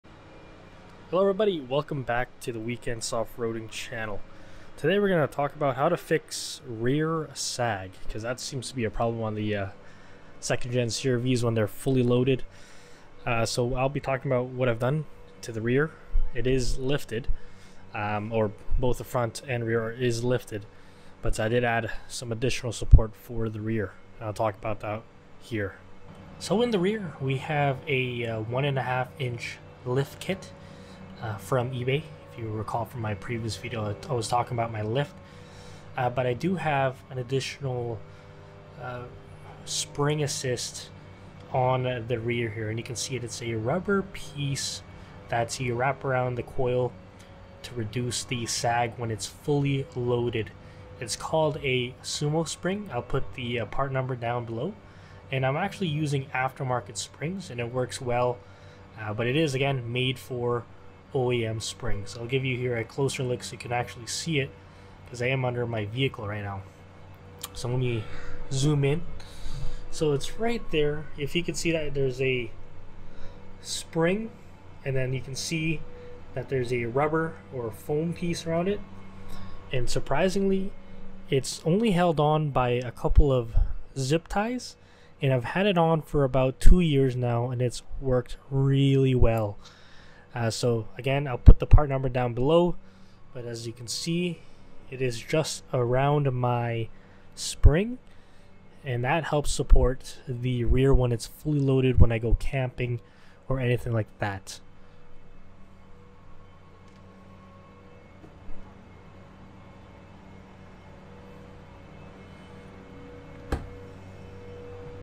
How to Fix REAR SAG on a 2nd Gen CRV
I've had my CRV for about 4 years now and ever since I got it the rear end always sagged with minimal weight. Today's video shows how I was able to fix the rear sag on a 2nd gen CRV while I'm fully loaded with camping and overland gear. This can apply to other vehicles as well.

Part Number: CSS-1125

Amazon says these don't fit the CRV but they do, you can double-check SumoSpring's Website.